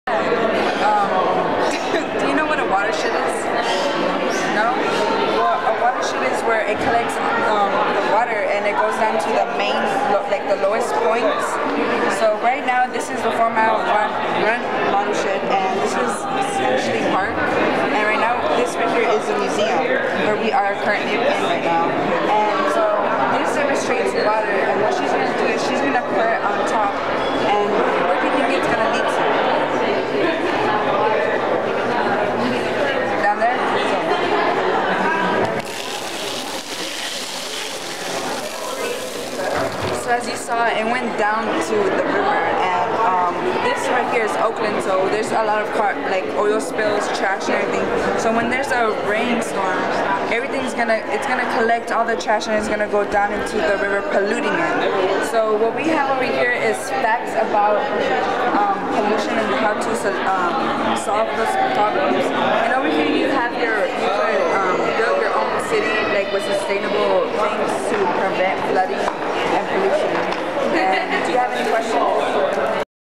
Watershed Model: Where water flows and goes
Energy Net teens explain the concept of a watershed. A watershed is an area of land that drains to a river or stream. The teens pour small beads over their watershed model to demonstrate how water flows downhill towards the river.